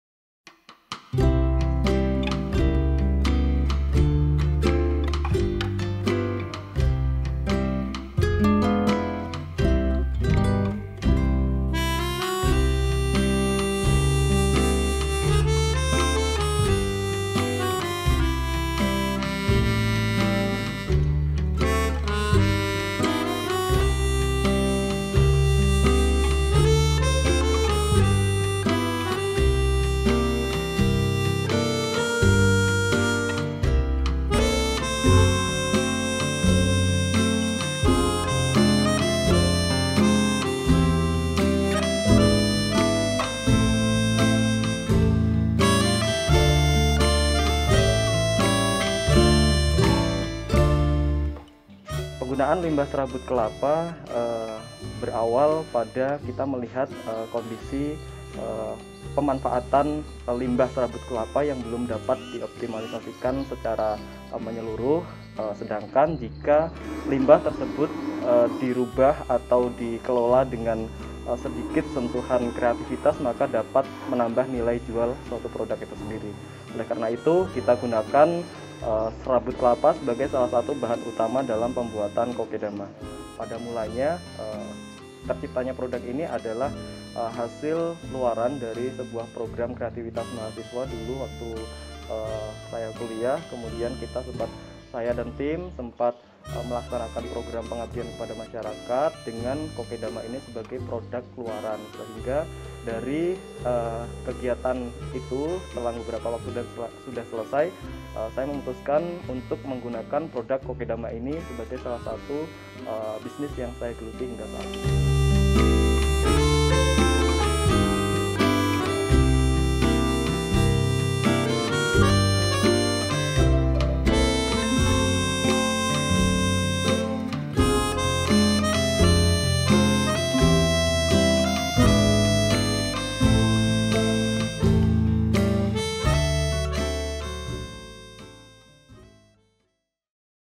Melirik Peluang Bisnis Kokedama Teknik Tanaman Hias dari Jepang
Seorang Santri pondok pesantren di kota Madiun, Jawa Timur, sukses mengembangkan media tanam dengan teknik Kokedama.